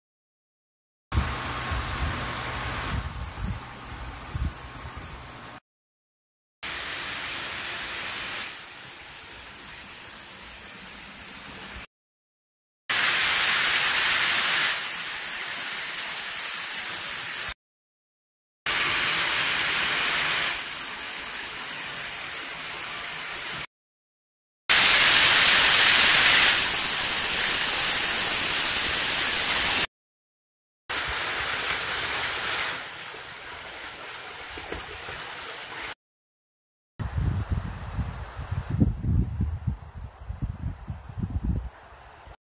[SHL25]東京都水の科学館 壁泉[4K] -The wall fountain in Tokyo Water Science Musium-
2014.Oct.4 東京都水の科学館(東京都江東区)の壁泉" The wall fountain in Tokyo Water Science Musium, Koto City, Tokyo, Japan.SHARP AQUOS SERIE SHL25.